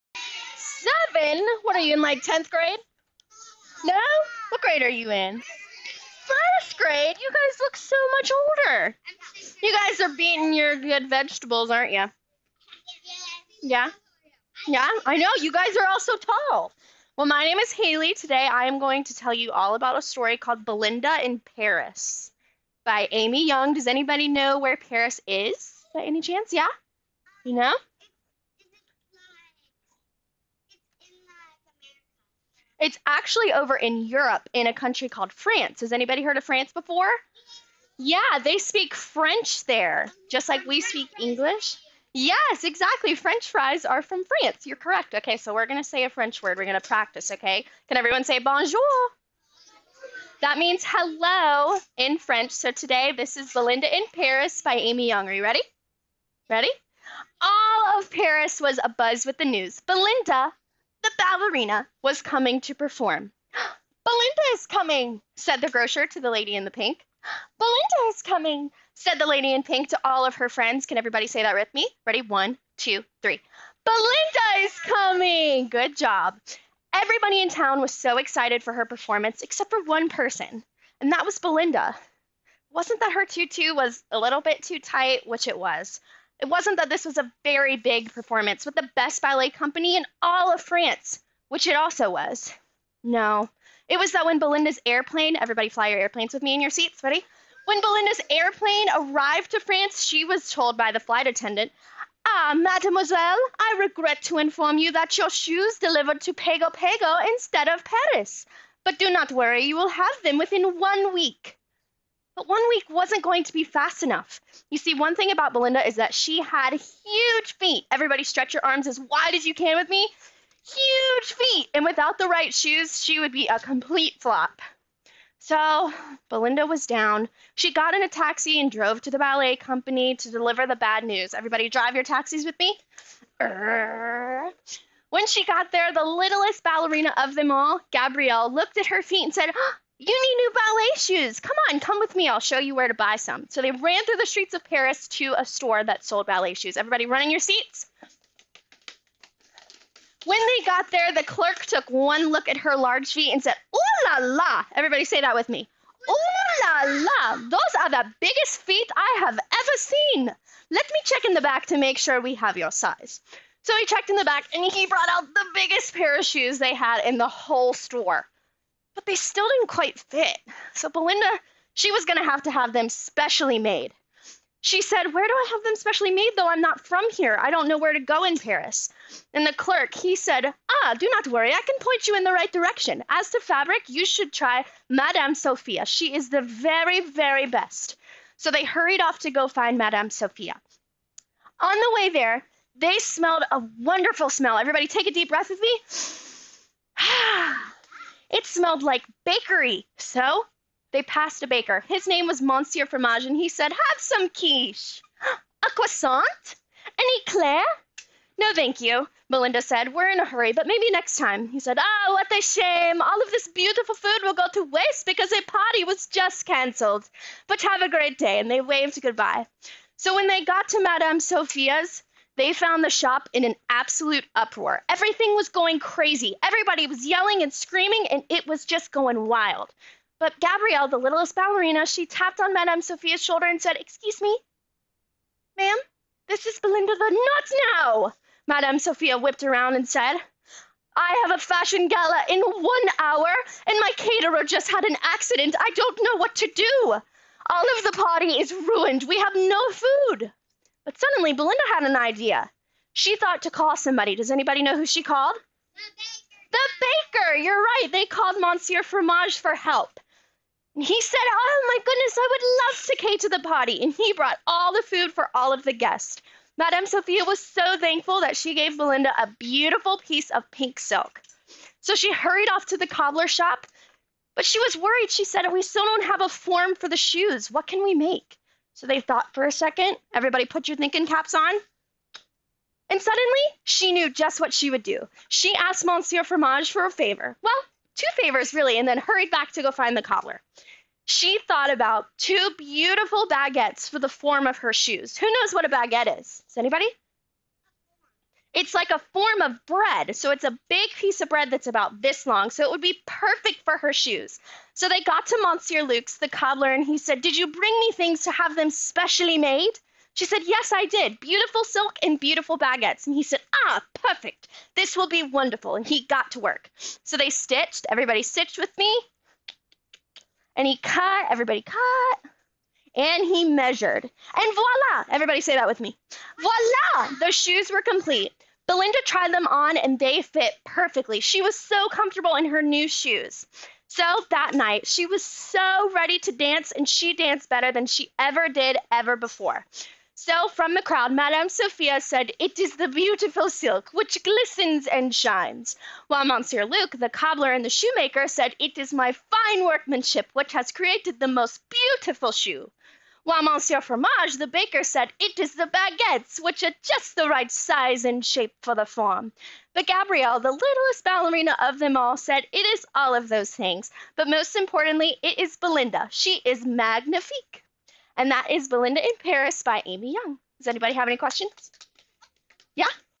Belinda in Paris by Amy Young performed by Haley Cissel!
This story was performed at Mary C Williams Elementary!